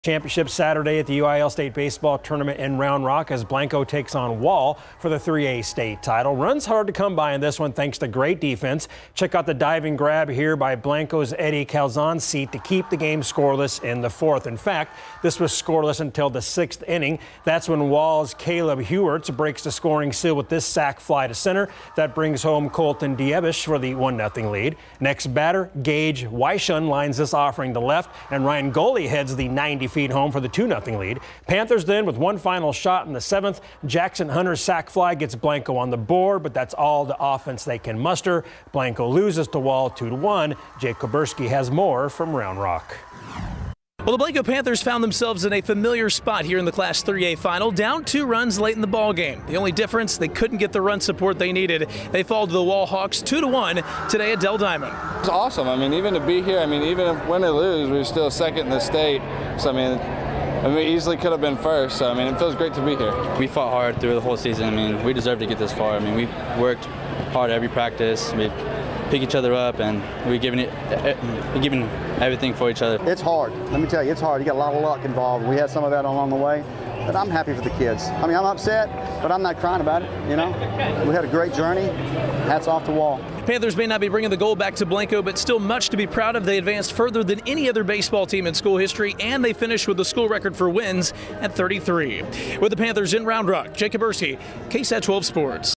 Blanco falls short in 3A state championship game
The Blanco Panthers fell to the Wall Hawks 2-1 on Saturday in the UIL Class 3A state championship game at Dell Diamond in Round Rock.